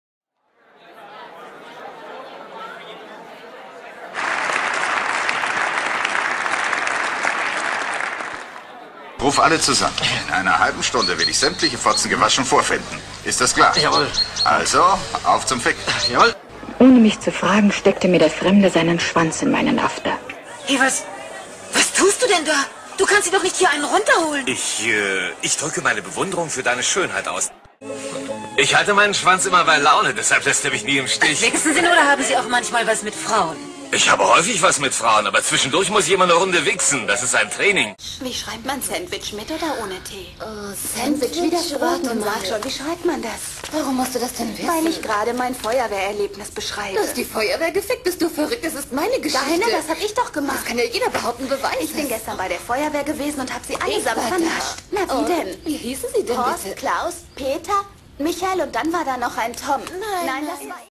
Dirty Bundestag - Porn Synchronisation, FUNNY
warum liegt hier stroh kennt man ja schon, hier eine andere art der verwendung solcher dialoge :)